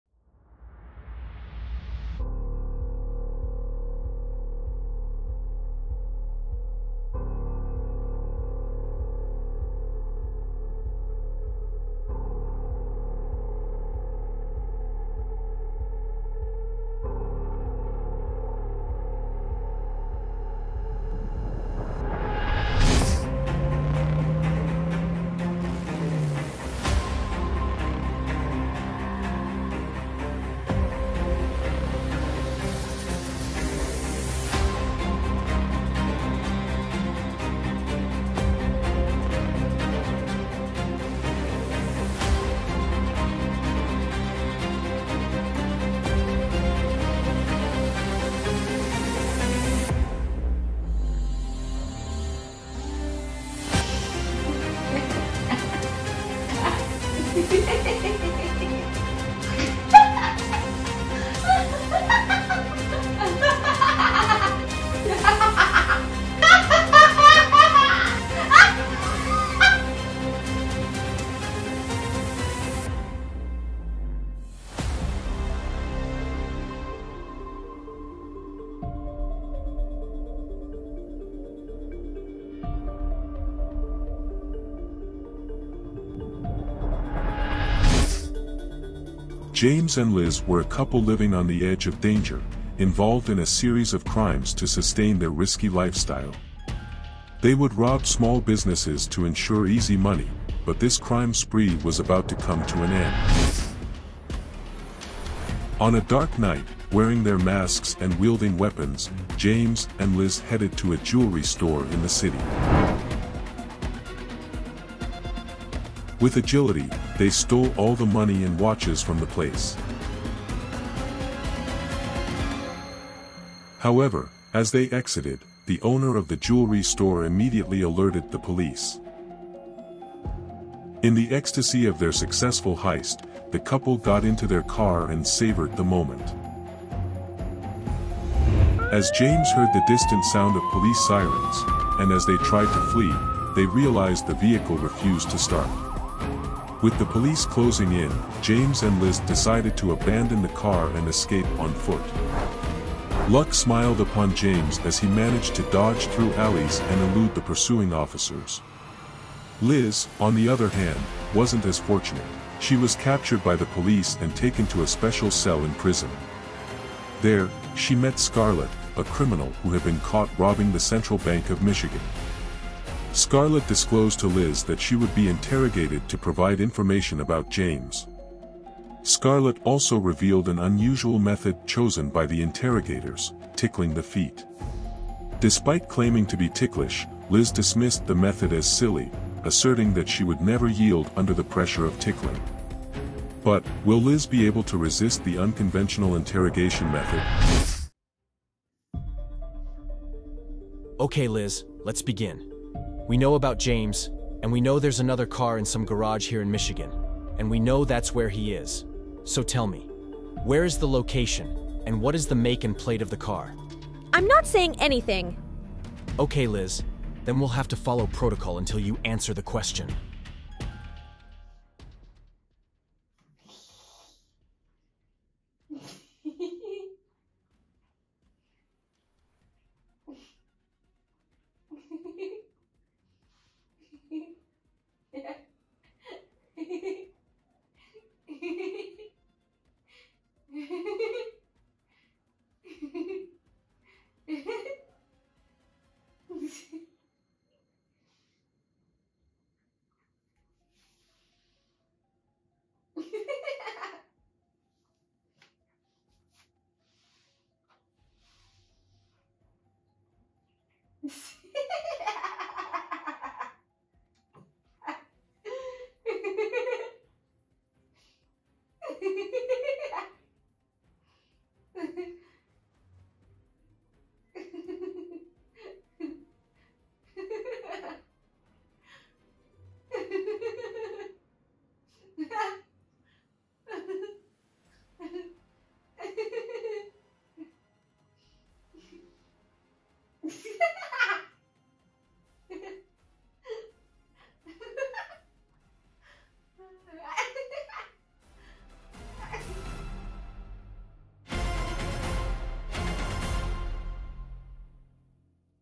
Captured Criminal Woman Liz Gets A Tickle Interrogation!
James and Liz were a couple living on the edge of danger, engaged in a series of crimes to sustain their risky lifestyle. They would rob small businesses to ensure easy money, but this crime spree was about to come to an end.